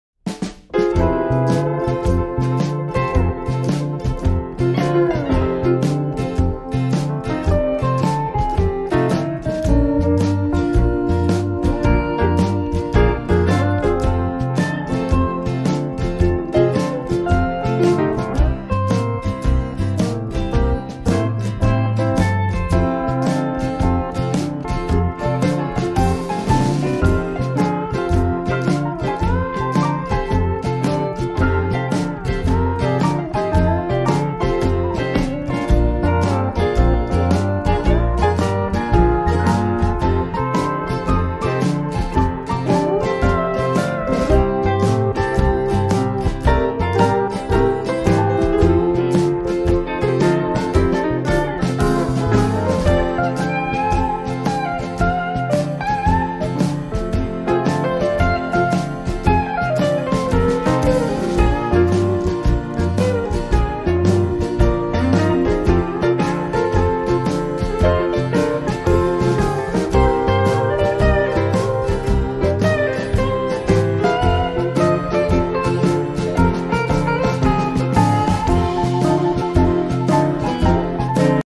Pastel Pet Portraits of Labrador Retriever.
Custom pet portraits of Labradors. Short demo video of custom portraits created by myself.
Don't hesitate to get in touch if you are thinking to commission a pet portrait. Posting worldwide.